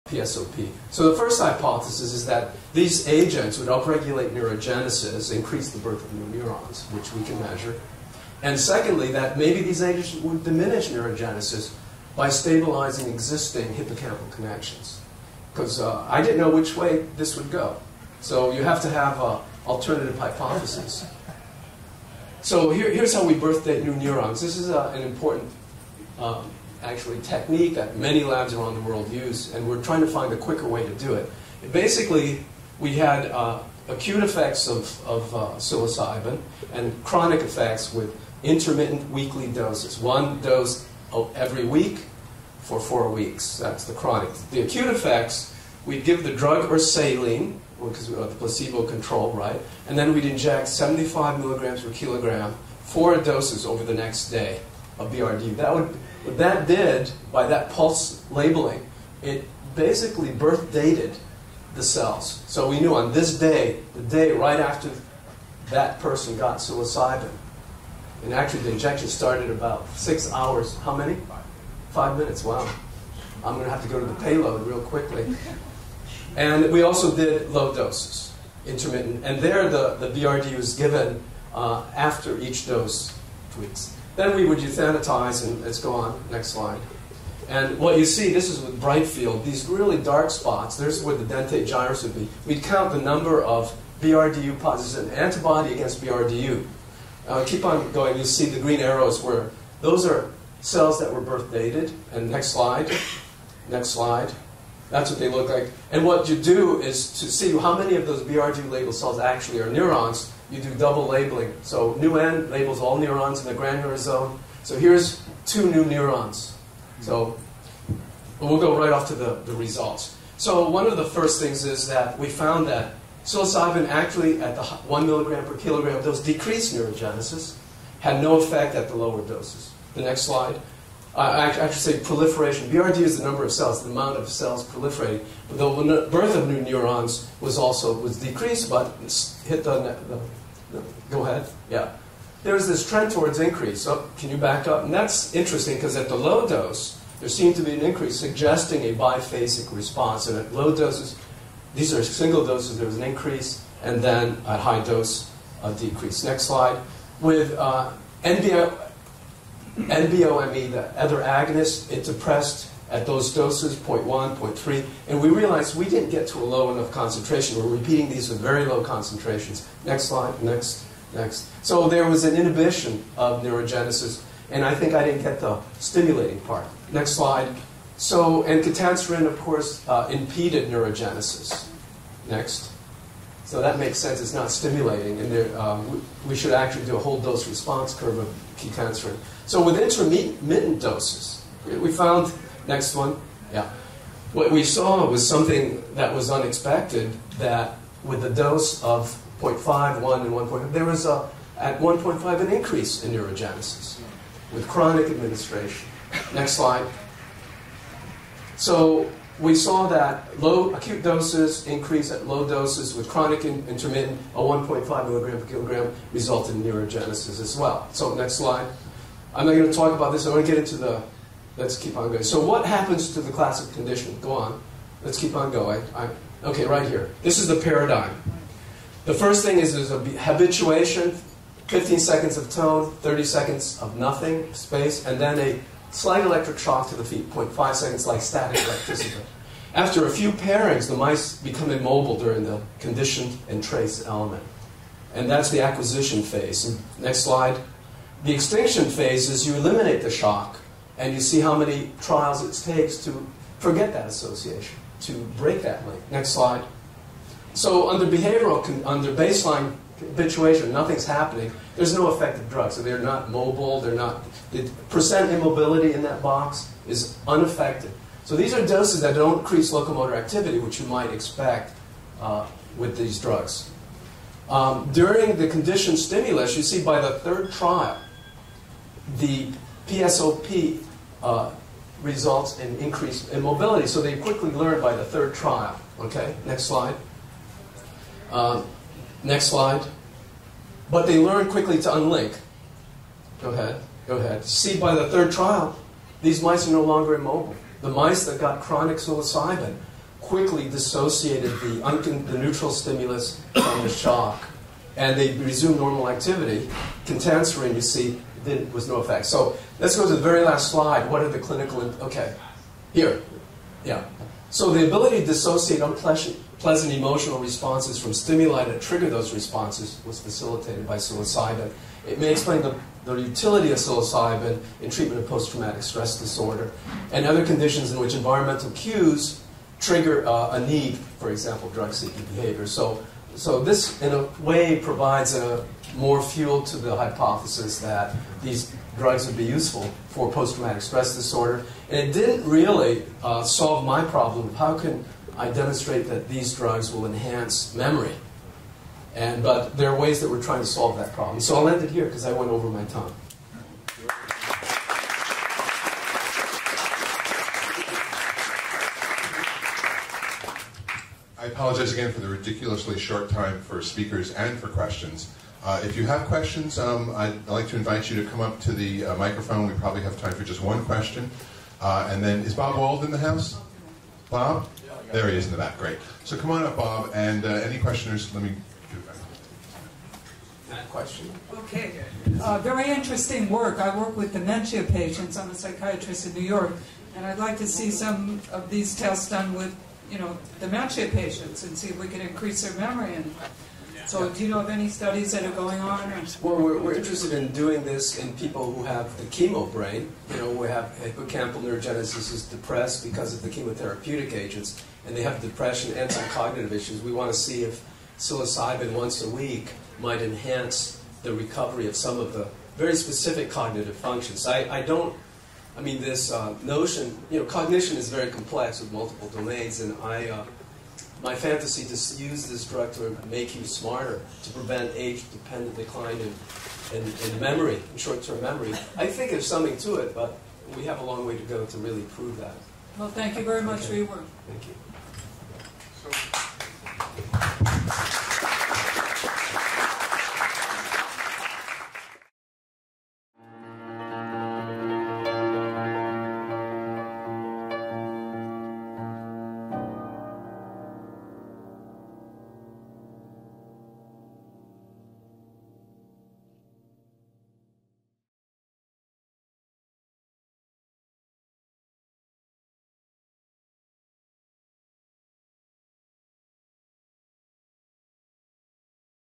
Psilocybin Research - Zeno Sanchez-Ramos - Part 2 of 2
Zeno Sanchez-Ramos studies the effects of psilocybin and other selective serotonin agonists on hippocampal-dependent learning and neurogenesis.